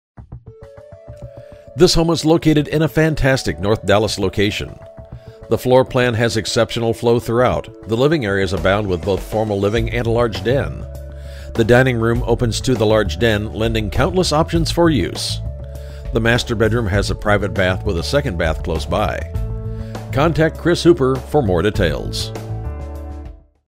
12540 Lochmeadows Drive Dallas, TX | ColdwellBankerHomes.com
12540 Lochmeadows Drive, Dallas, TX

Fantastic North Dallas location! The floorplan has exceptional flow throughout the home. Living areas abound with both formal living and a large den with a beautifully centered fireplace. The dining room opens to the large den lending countless options for use. The master bedroom has a private bath